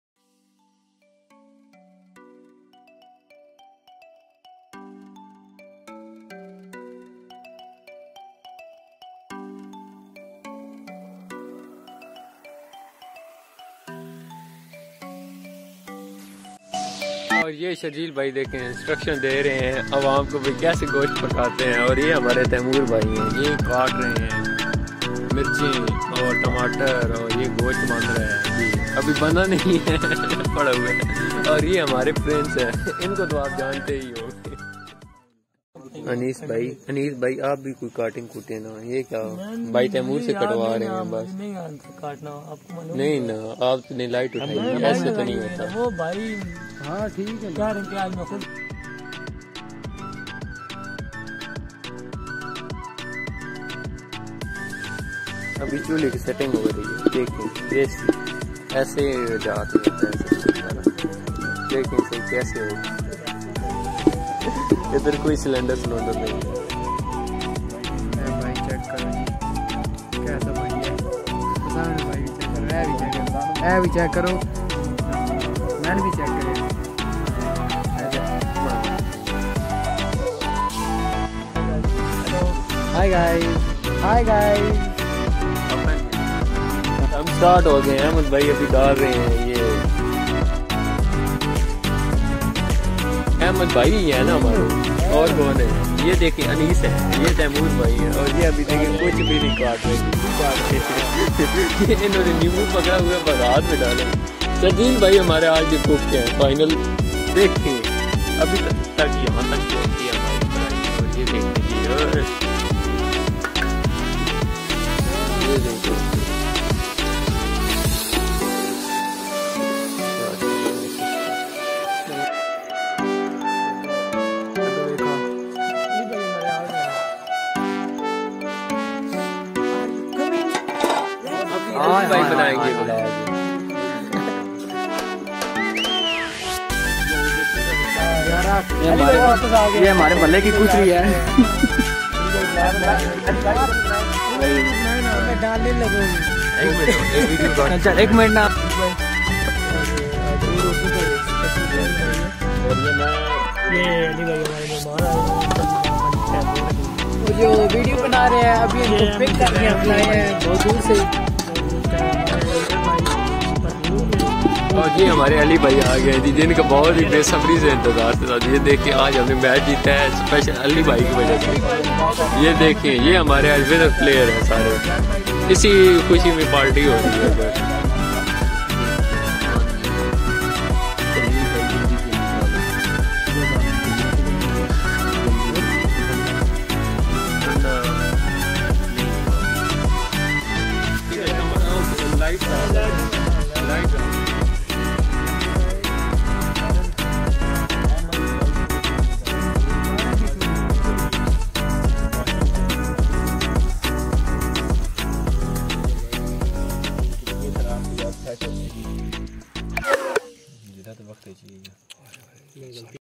Hi Mazay😂We're having a party! Karhai Party With Friends
night Party With Friends
krahi party with friends

Zain islam
zainz96